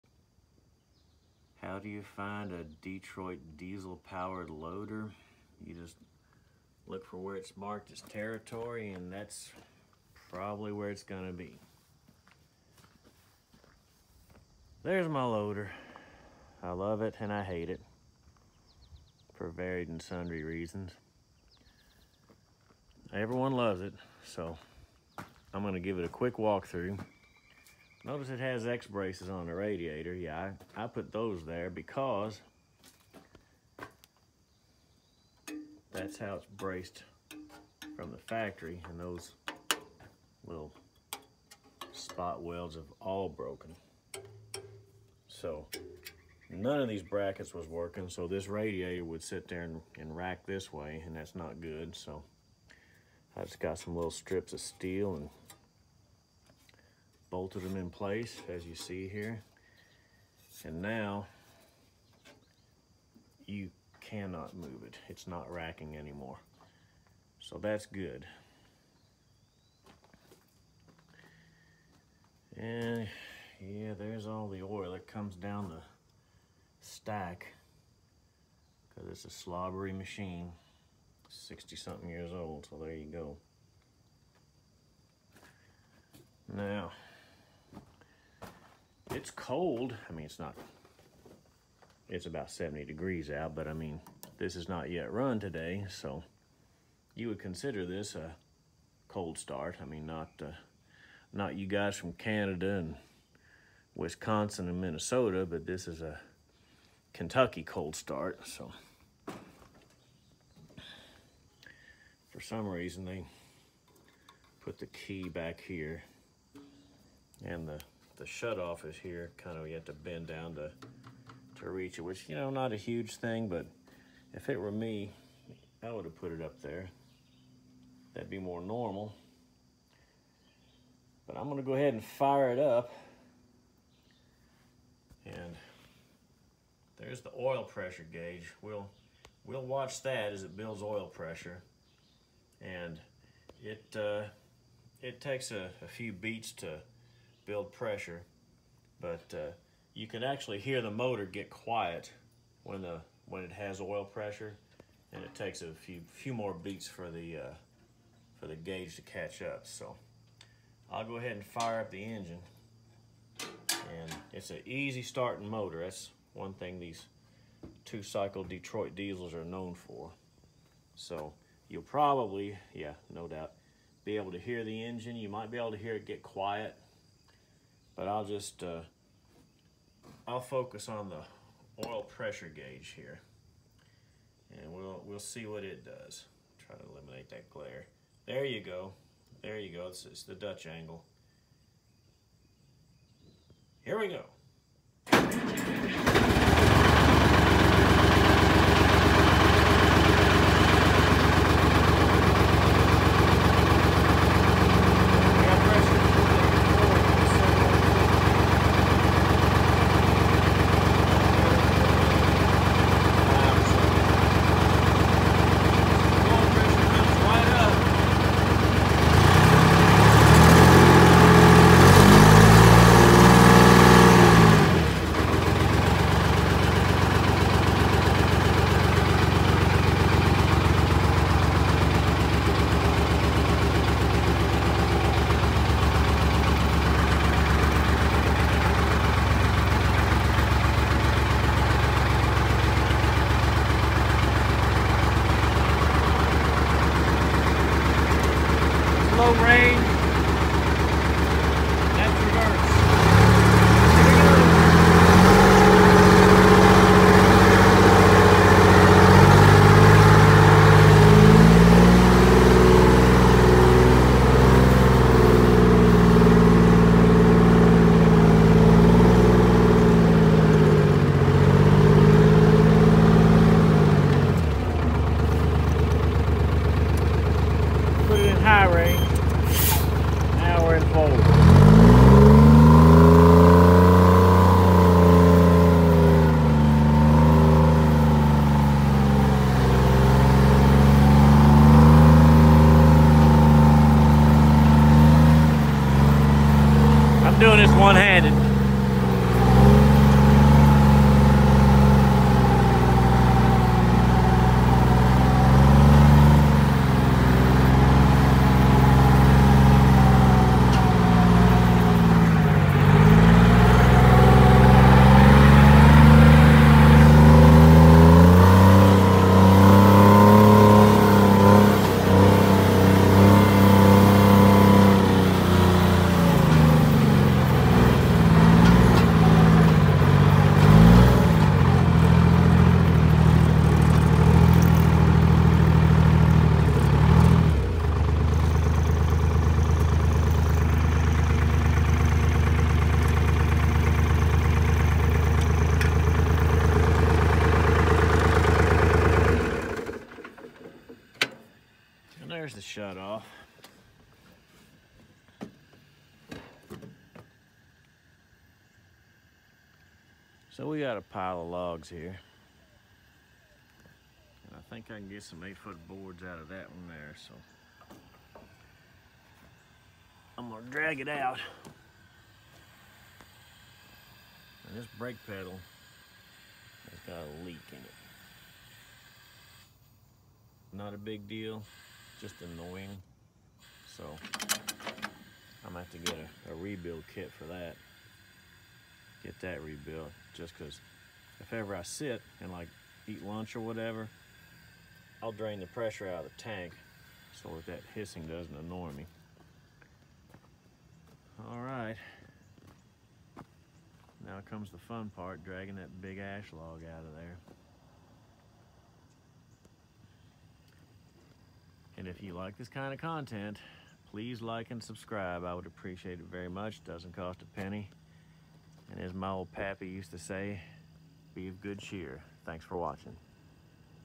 How do you find a Detroit Diesel powered loader? That’s easy!
It sure is loud.